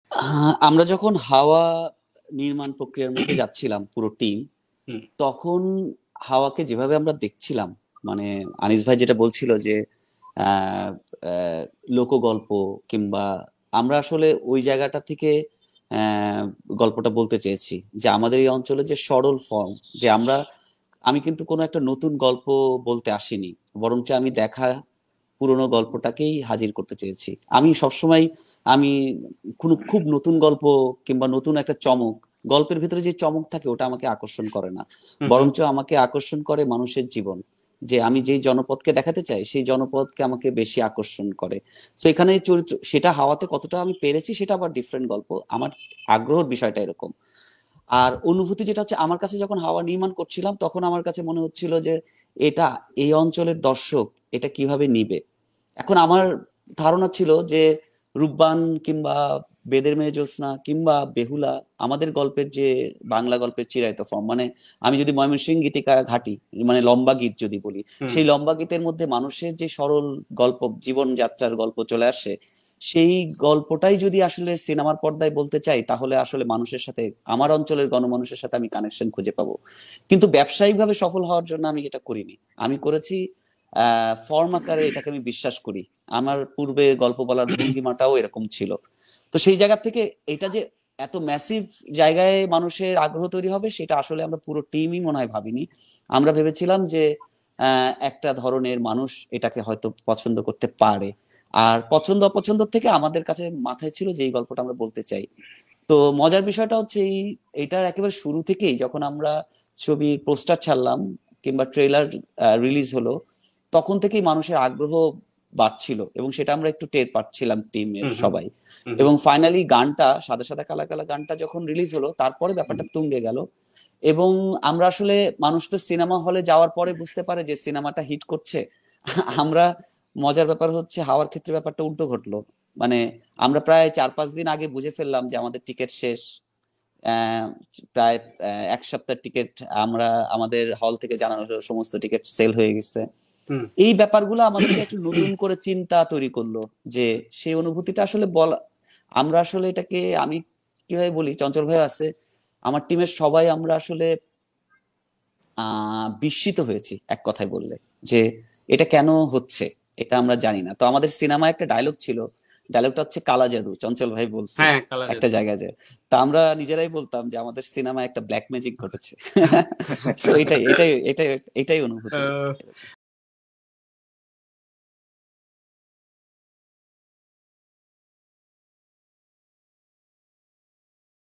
”’হাওয়া’য় কালা যাদু ভর করেছিল“: মেজবাউর রহমান সুমন
গত ৭ই জুলাই ভারতীয় প্লাটফর্ম সনি লিভ-এ মুক্তি পাবার পর ‘হাওয়া’ চলচ্চিত্র নিয়ে আবার আলাপ হচ্ছে, এবার পশ্চিমবঙ্গের বাইরের ভারতে। ‘হককথা’য় অতিথি হিসেবে উপস্থিত হয়ে ’হাওয়া’ নিয়ে কথা বলেছেন পরিচালক মেজবাউর রহমান সুমন।